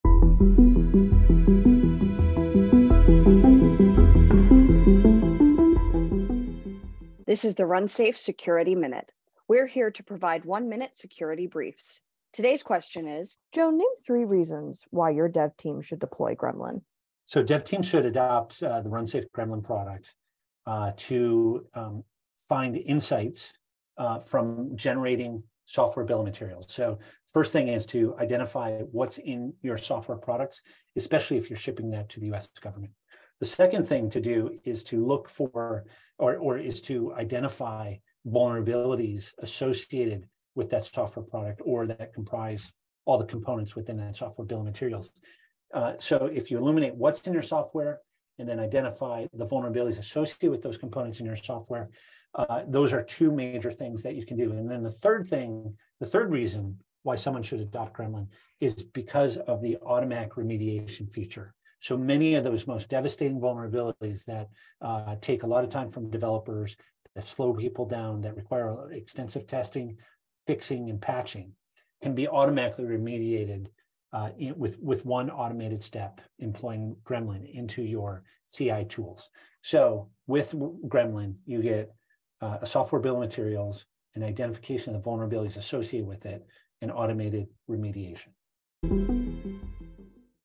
"Name three reasons your dev team should deploy Gremlin" with Joe Saunders
Find out three reasons your dev team should deploy Gremlin in this week's RunSafe Security Minute with our very own founder/CEO, Joe Saunders!